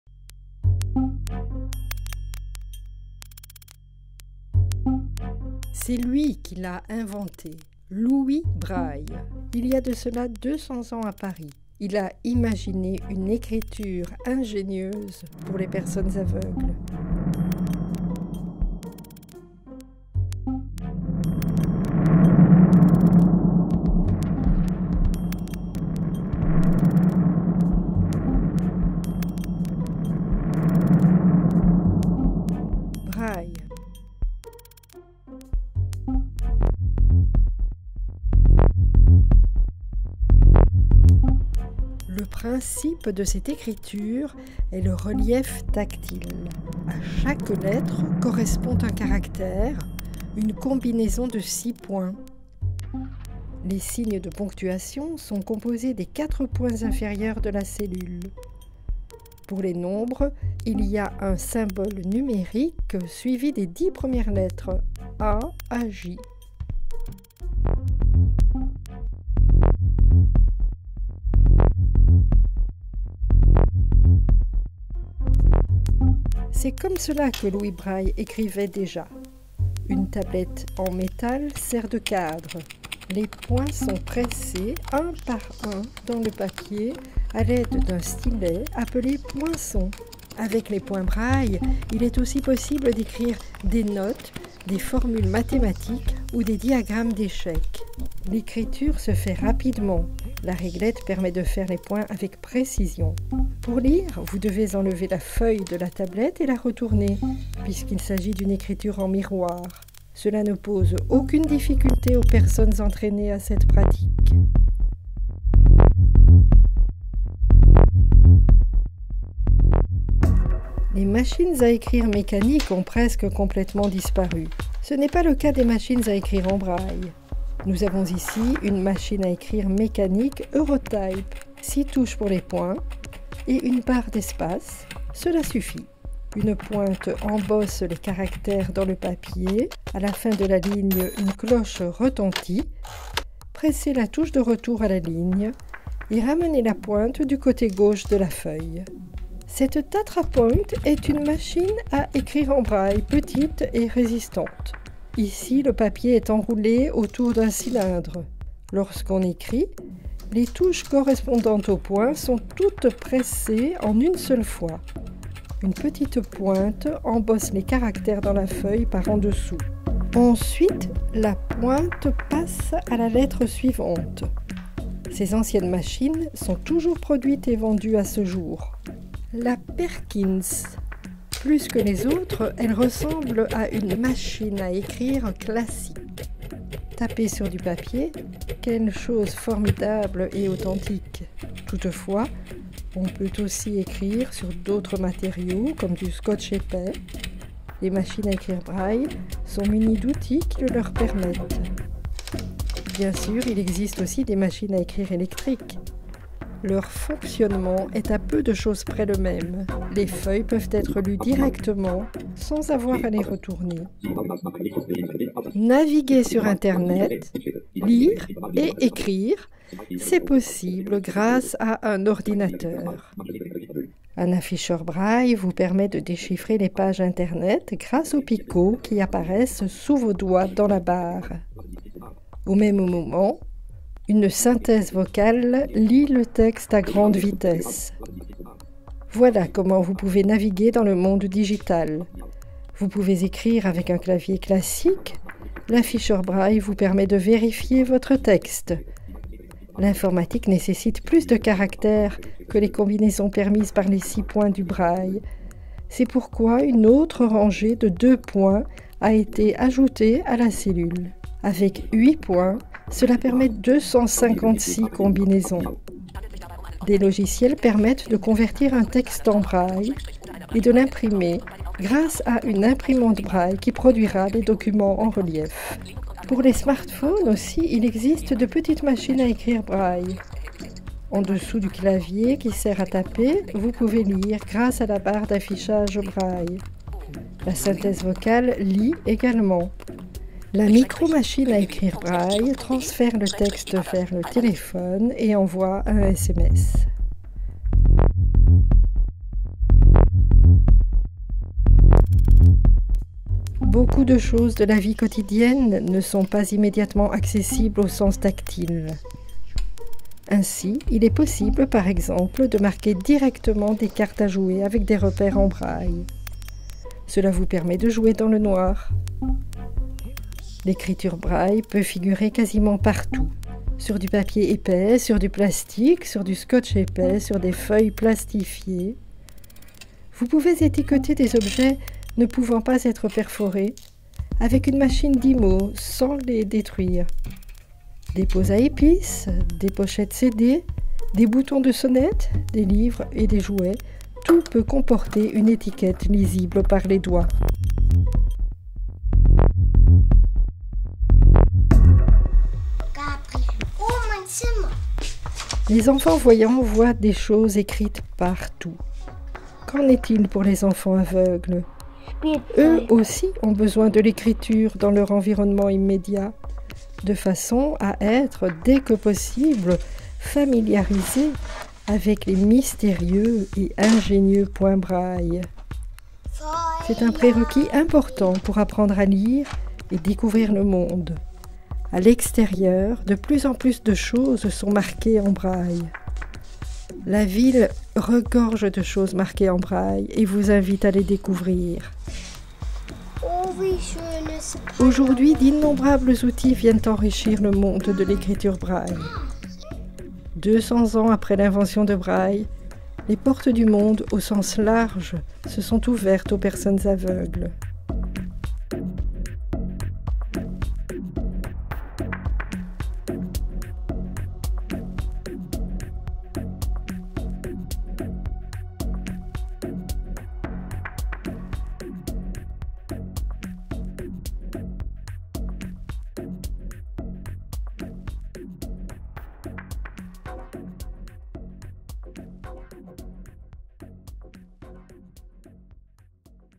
Braille Un brillant écrivain
C'est lui qui l'a inventee. Louis Braille, il y a de cela 200 ans ä Paris, il a imagine une
ecriture ingänieuse pour les personnes aveugles. C'est comme cela que Louis Braille ecrivail^dejä. Une tablette en metal sert de
II cadre, les points sont presses un par un dans le papier ä l'aide d'un stylet appele
/ poingon. Avec les points Braille il est aussi possible d'ecrire des notes, des formules
mathematiques ou des diagrammes d'echecs. L'ecriture se fait rapidement. La
regierte permet de faire les points avec precision